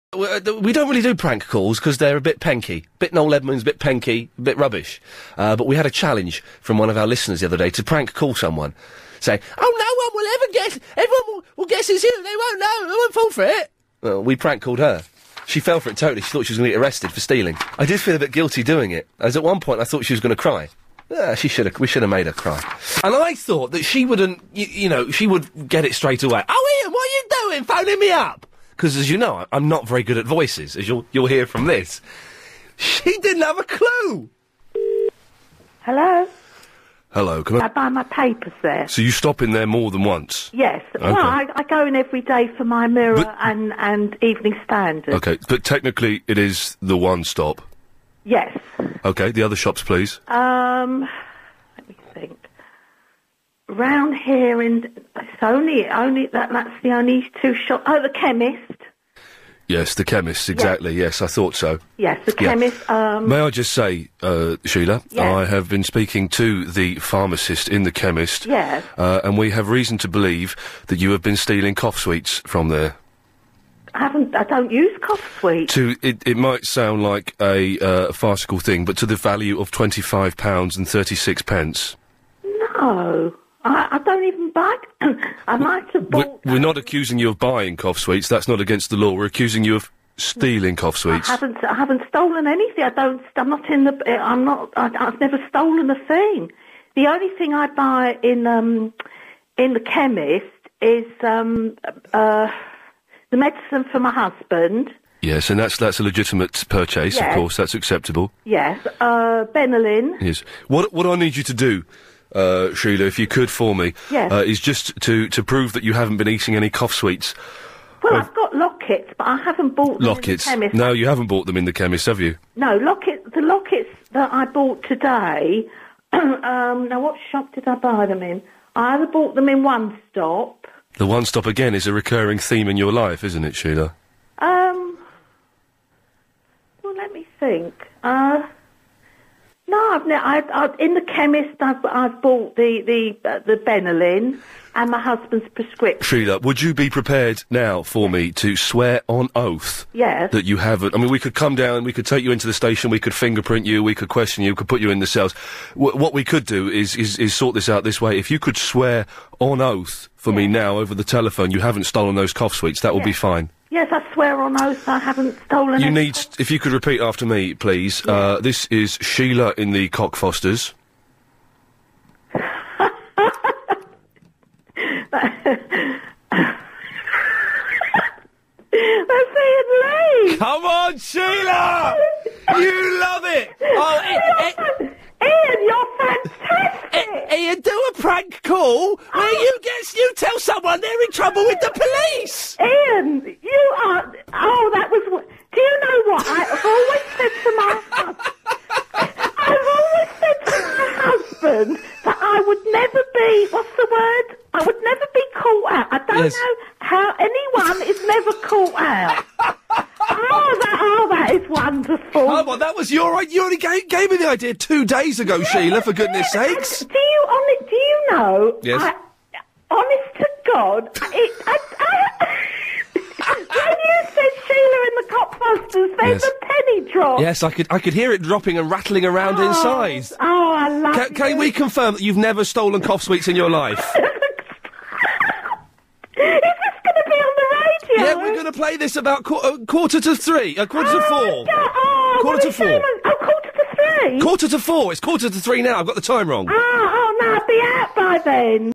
LBC: Prank Call
Iain Lee prank calls Sheila who said that she'd never fall for a prank call... well she was wrong!

I don't know what happens 37 seconds in, a few seconds are lost - basically Iain 'Police Officer' Lee asks her what shops she's been to recently.